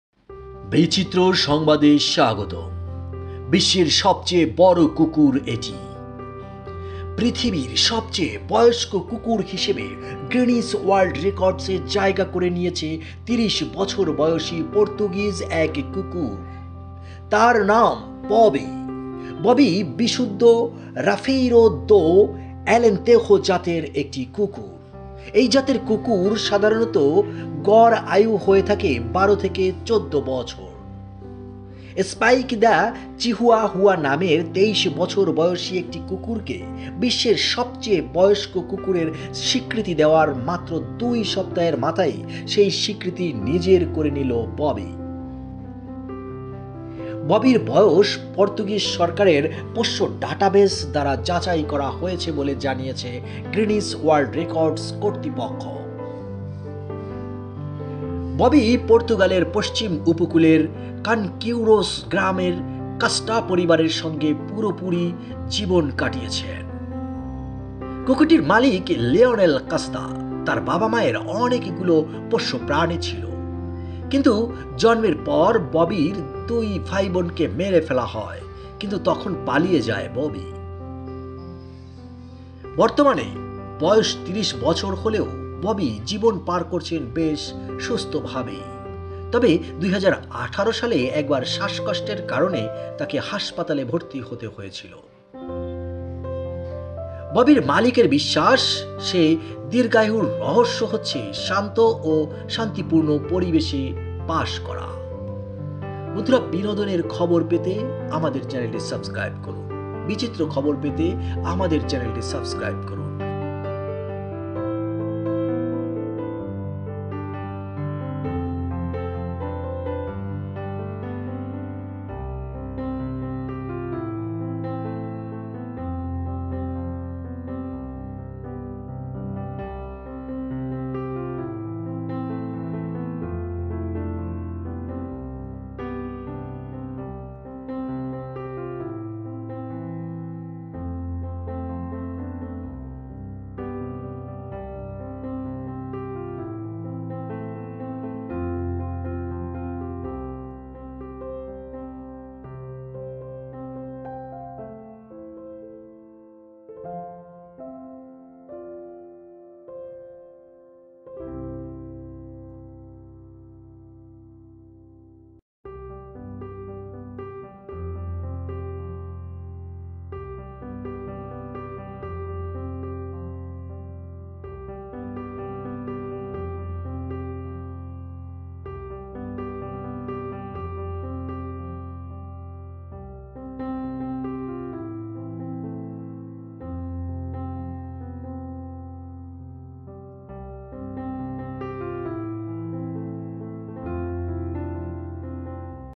বিশ্বের সবচেয়ে বয়স্ক কুকুর এটি, কত বছর জানেন | Bobi breaks Guinness World Record for oldest dog ever
বিশ্বের সবচেয়ে বয়স্ক কুকুর এটি | Bobi breaks Guinness World Record for oldest dog ever.